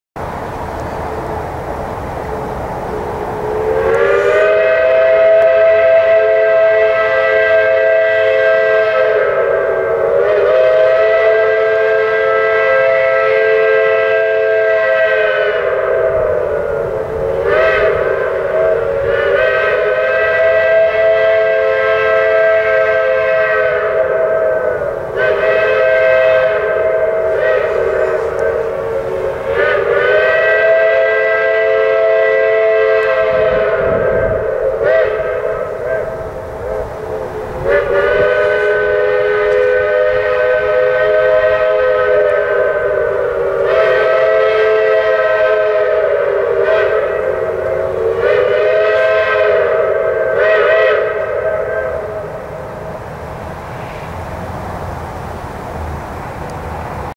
My Favorite Train Whistle - (CROSBY WHISTLE) during Nov 2014 Youngstown Ohio Train Whistle Blow
Here is video I recorded back in November 2014 in Youngstown Ohio of a Crosby Train Whistle during the Youngstown Train Whistle Blow of 2014.

Video link to all the whistles I recording during this, here is the link to Part 1